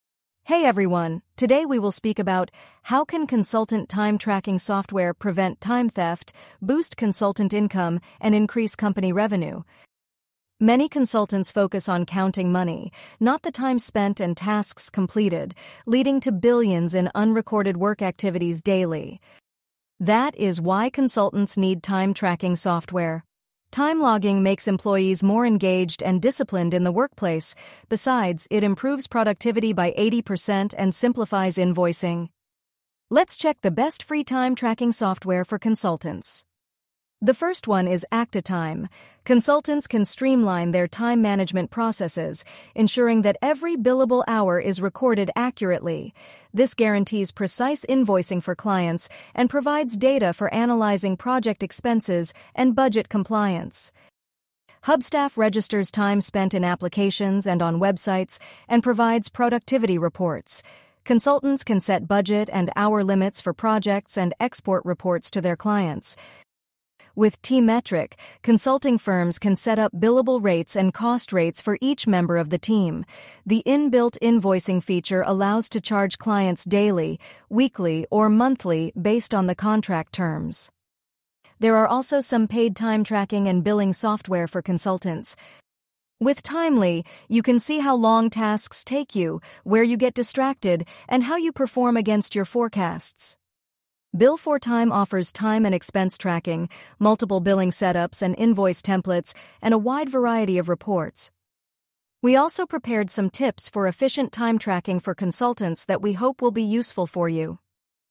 Best Time Tracking Software for Consultants
Most consultancies and self-employed consultants make the same mistake regarding productivity: they count money, not time spent or tasks completed. As a result, consultants often work extra hours, leaving their additional time unrecorded and unpaid.

In this video, we discuss how consultant time tracking software can address time theft and help increase company revenue.